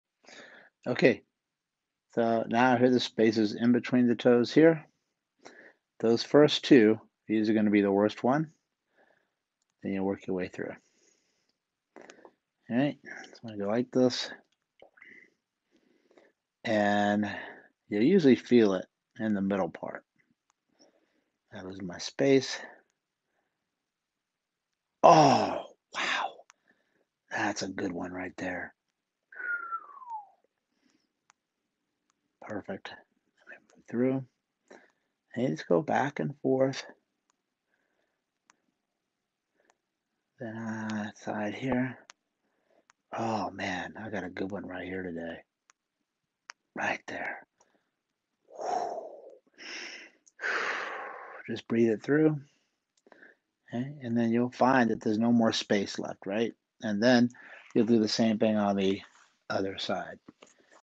Power of the Pencil Eraser #2
Work on top of the foot between the spaces with the eraser. DO BOTH FEET! May help foot, ankle, knee, hip, back, and walking. Do daily until it feels better. May take several weeks to totally go away in tenderness.